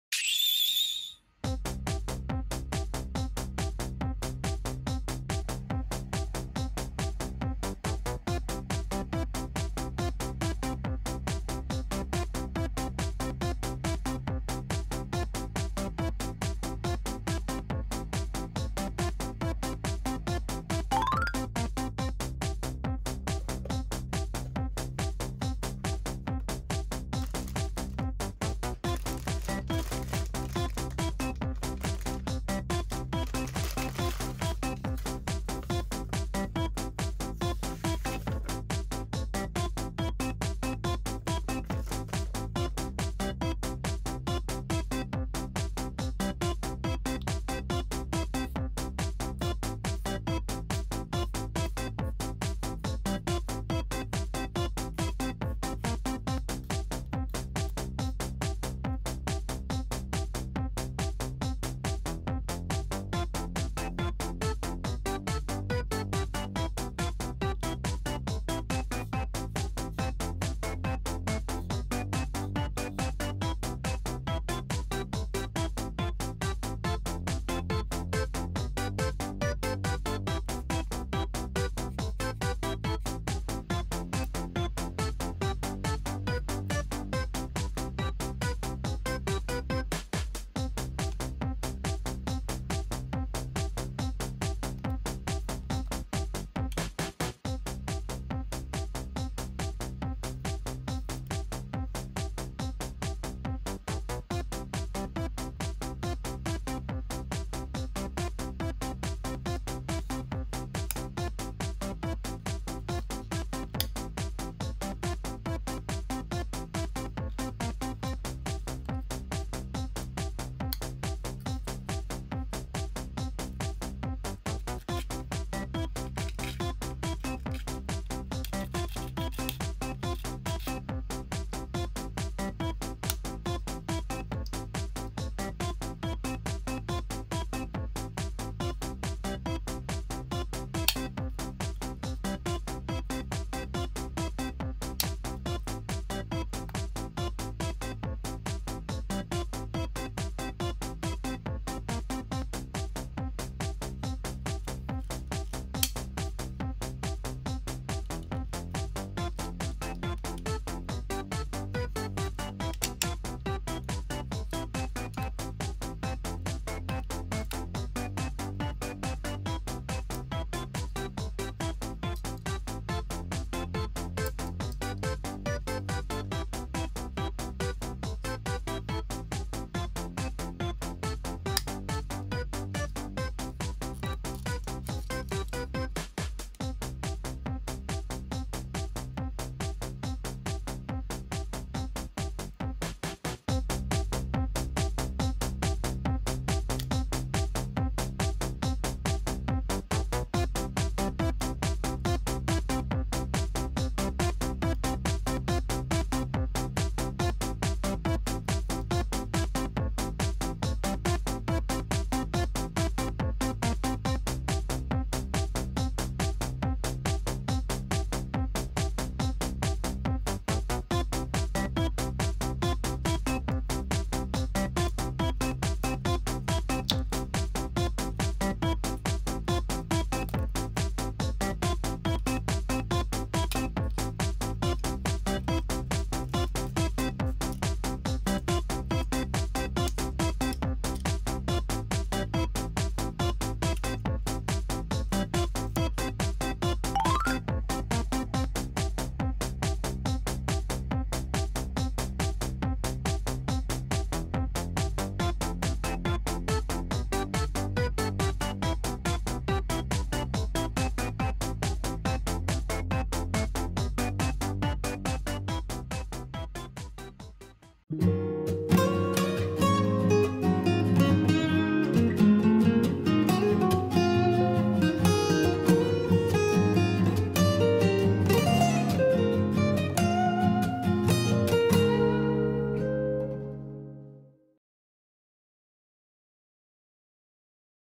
026 SDガンダムBB戦士No.14 ザクⅢ（ドダイ改つき）
SDガンダムBB戦士No.14　機動戦士ガンダムZZ
ザクⅢ（ドダイ改つき）
1988年8月発売

▶チャプター
00:00 スタート
00:21 開封
00:44 腕
01:14 頭
01:50 本体
02:49 ドダイ改
03:36 バズーカ
04:07 完成